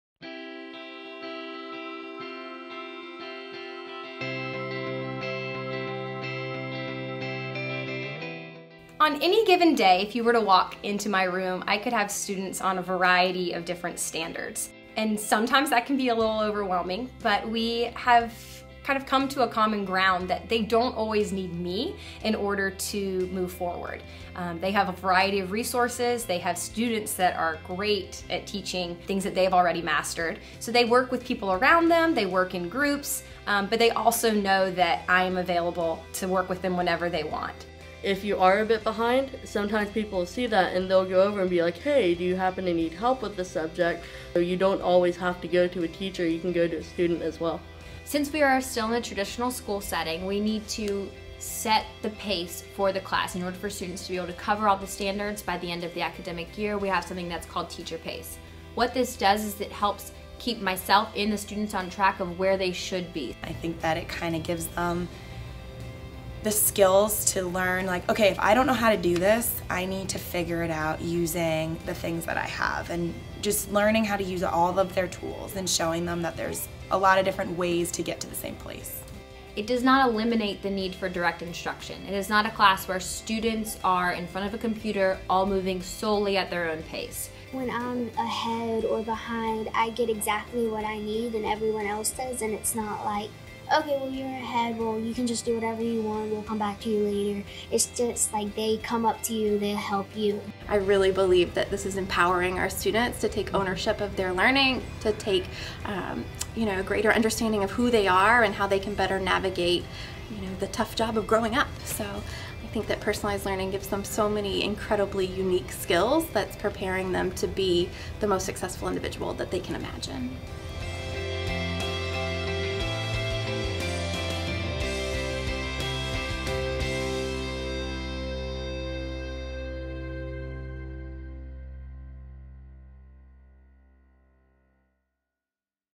Personalized Learning in South Carolina: Instructional Pacing
Instructional pacing in personalized learning is based on individual student needs and is calibrated to keep students on track for graduation and college-and-career-readiness.